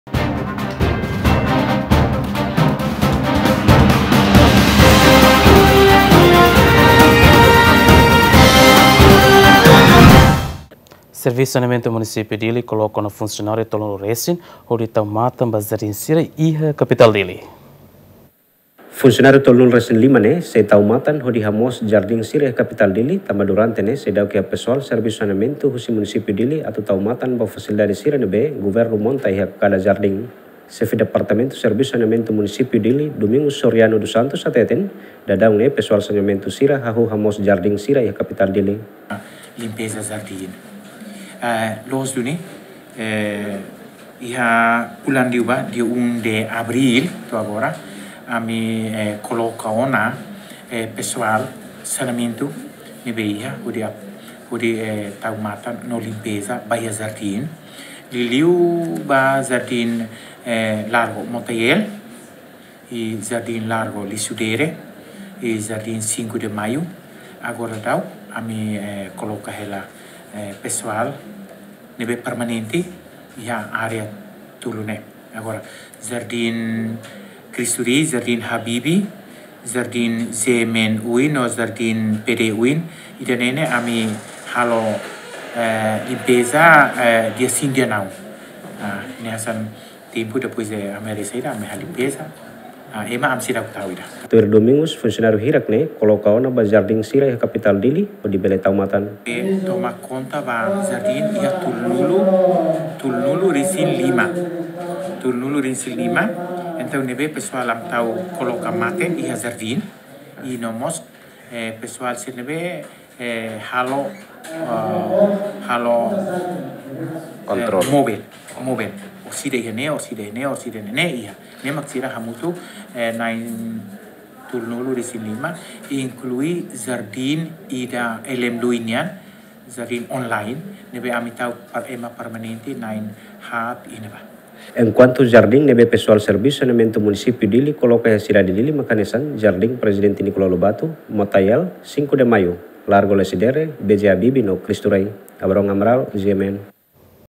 Servisu Saneamentu Díli koloka ona funsionáriu 30 resin hodi tau matan Jardim sira iha Kapital Díli
Servisu Saneamentu Munisípiu Díli koloka ona funsionáriu 30 resin hodi tau matan ba Jardim sira iha Kapital Díli